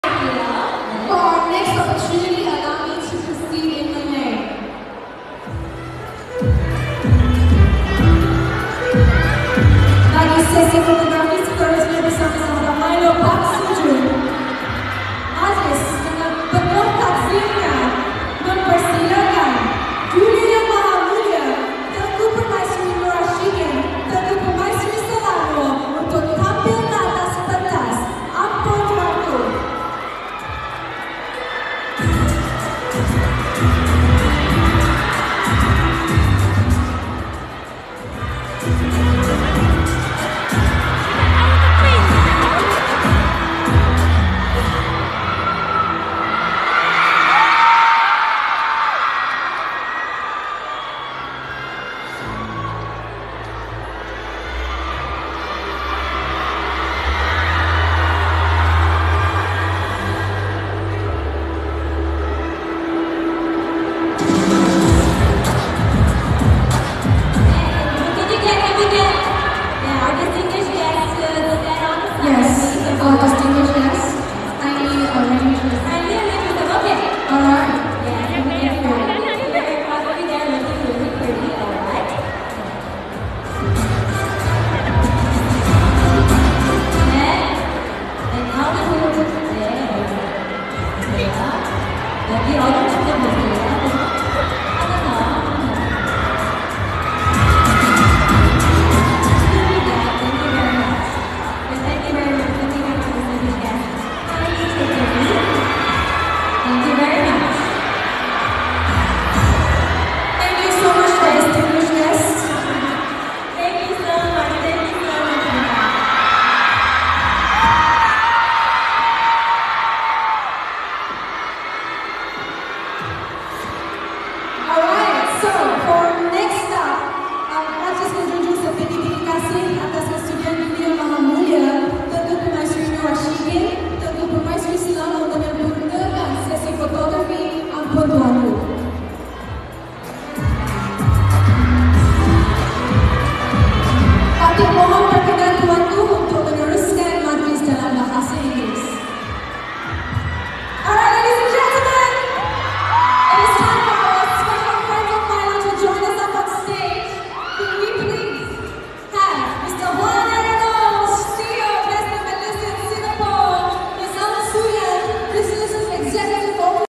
Park Seo Jun Sahabat MILO
Duta MILO Park SeoJun bergambar dengan tetamu kehormat MILO iaitu Tengku Permaisuri Selangor.